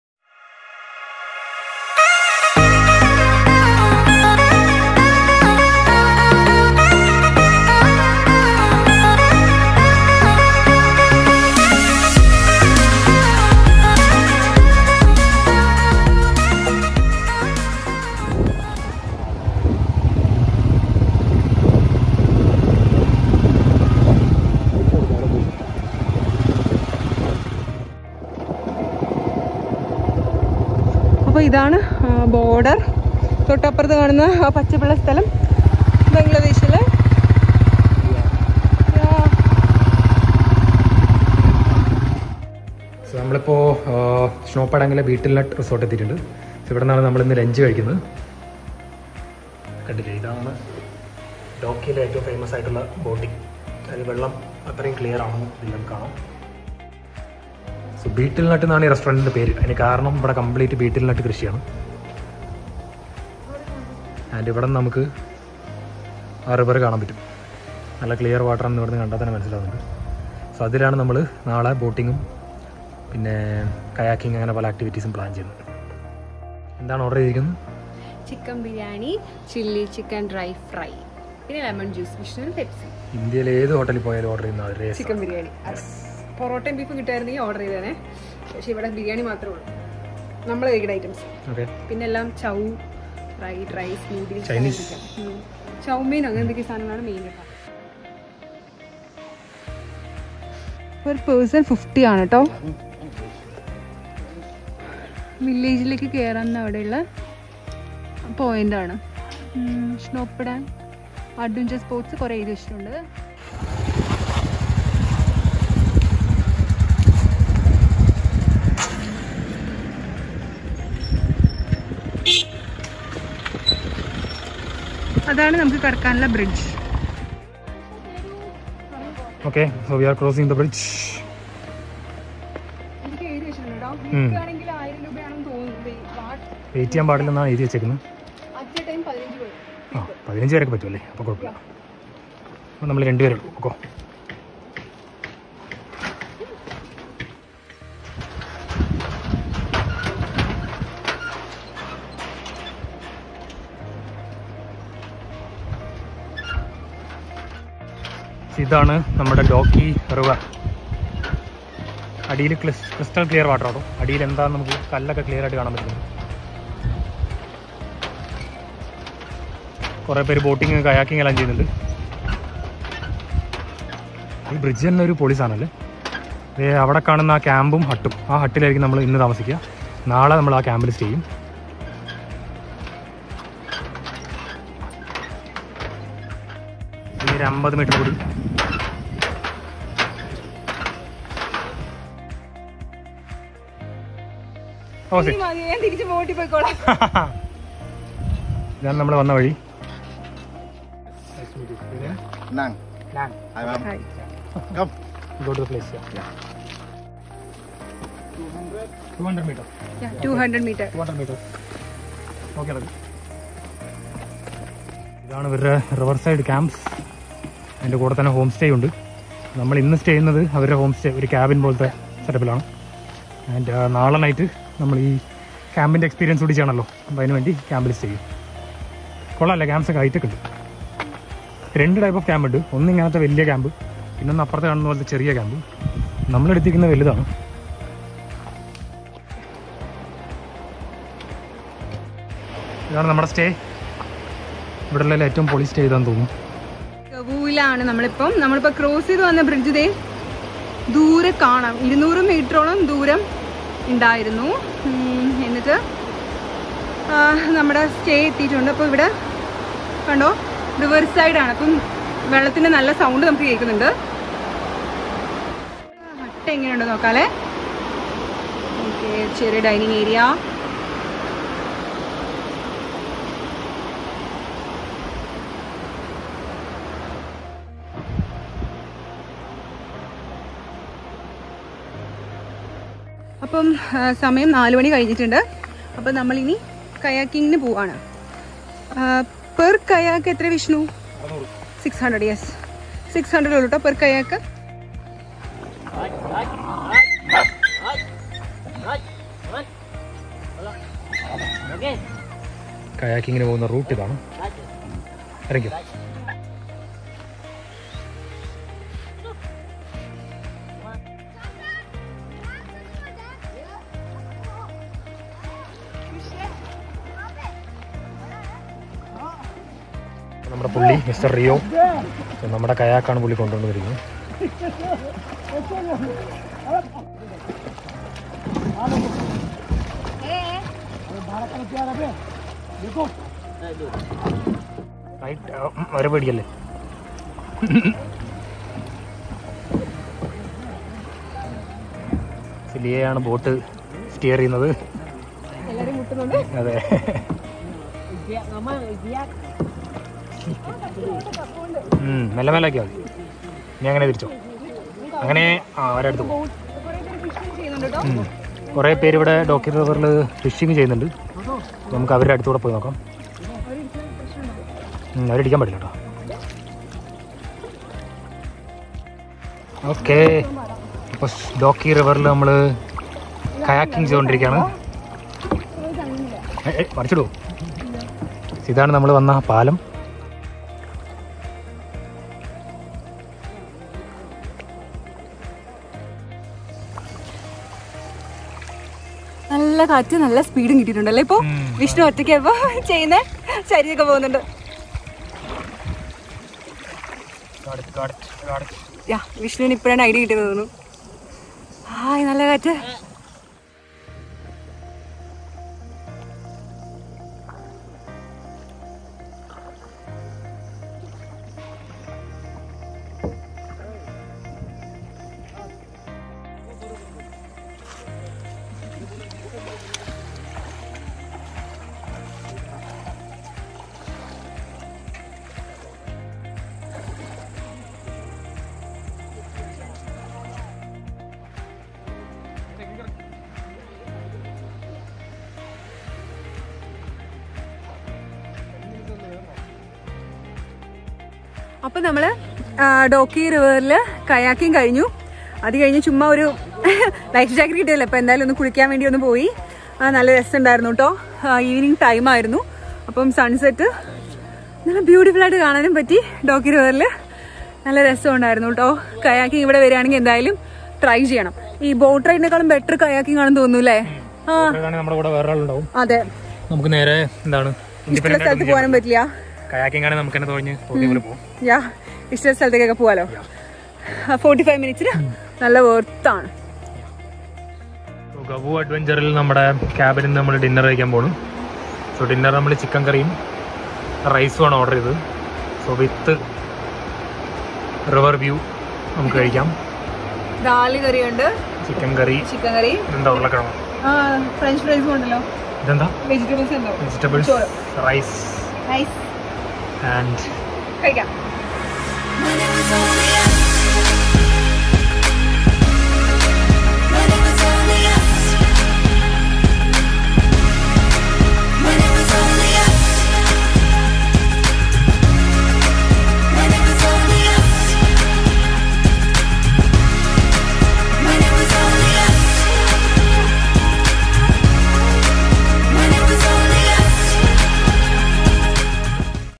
Dawki - Shnongpdeng | Kayaking - Gawooh | Meghalaya Vlog Part 5 | Malayalam Vlog | Liya & Vishnu
The fifth part of our Meghalaya Vlog is here. This video covers our days at Shnongpdeng. Our stay and activities at Shnongpdeng were arranged by Gawooh Adventure.